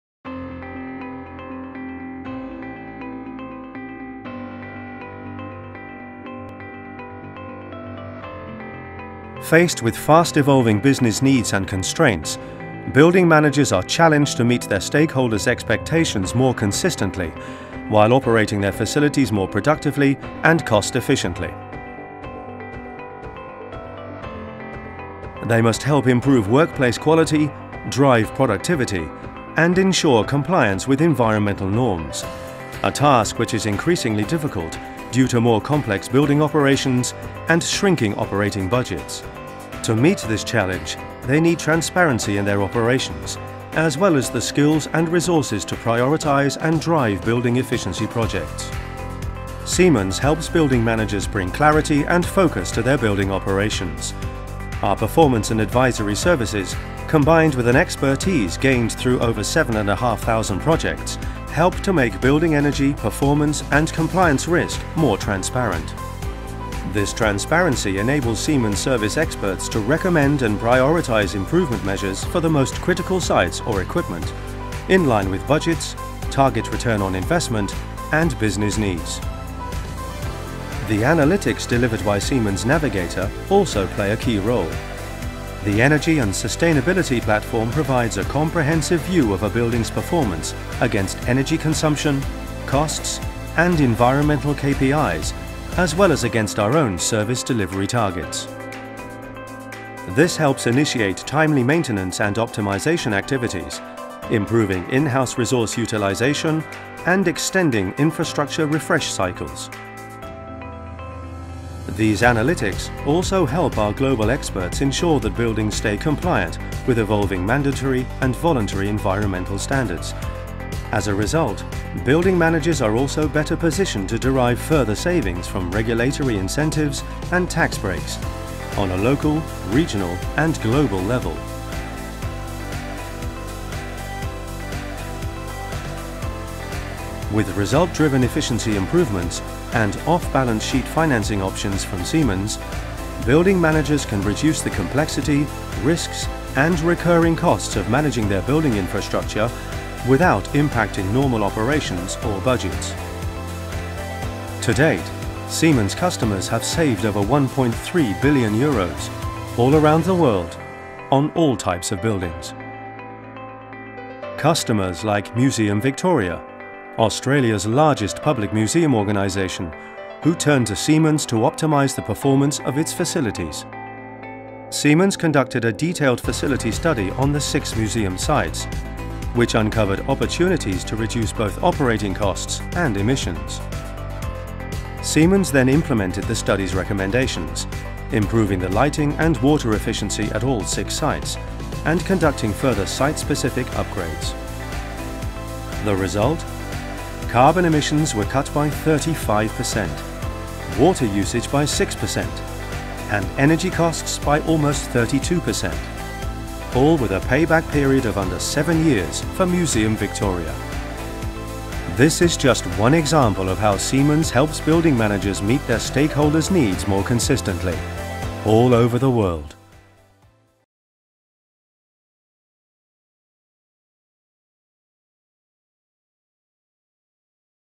Meeting stakeholders' needs more consistently through building performance optimization
Faced with fast evolving business needs and constraints, building managers are challenged to meet their stakeholders’ expectations more consistently, while operating their facilities more productively and cost-efficiently. Siemens helps Building Managers bring clarity and focus to their building operations. Our performance and advisory services, combined with an expertise acquired gained through from over 7500 projects, provide help create transparency into building energy, performance and compliance risks.